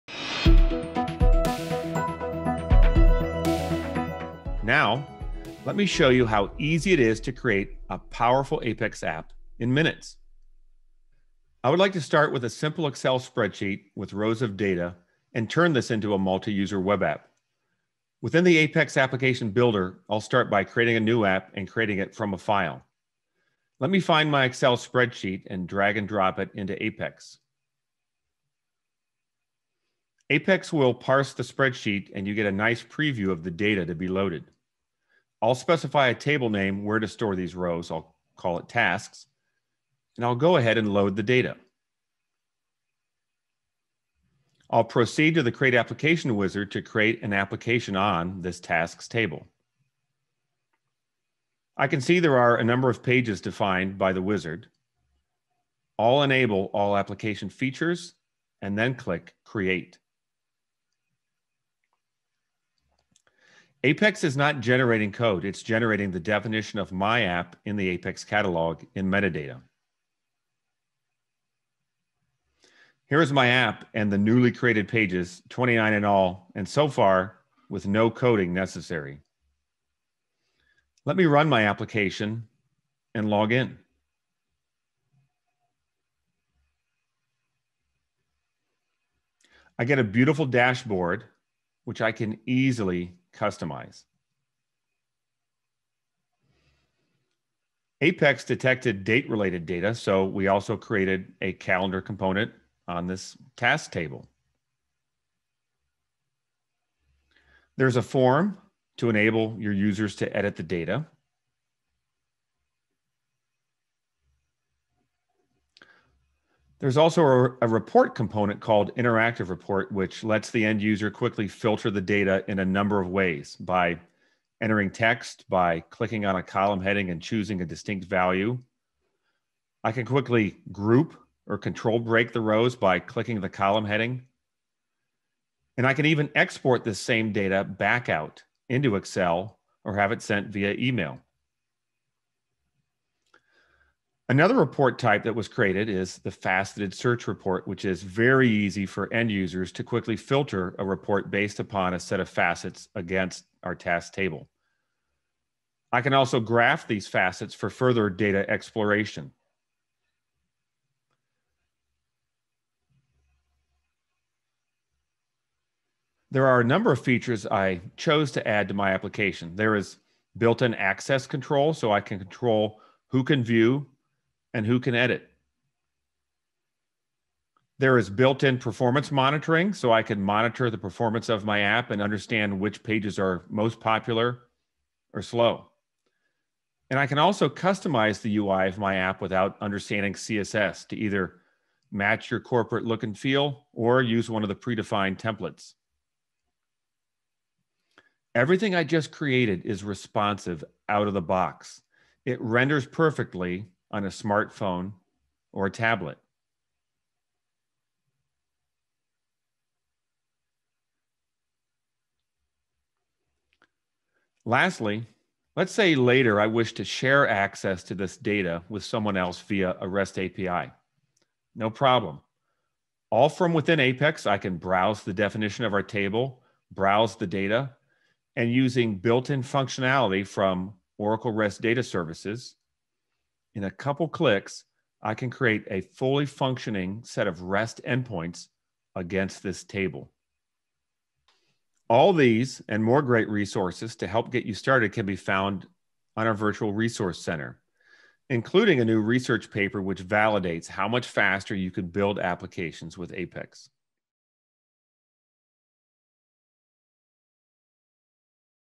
Getting Started with APEX
Joel Kallman, Senior Director for Oracle APEX,  shows how easy it is to get started with APEX, and demonstrate the creation of a powerful APEX app in minutes.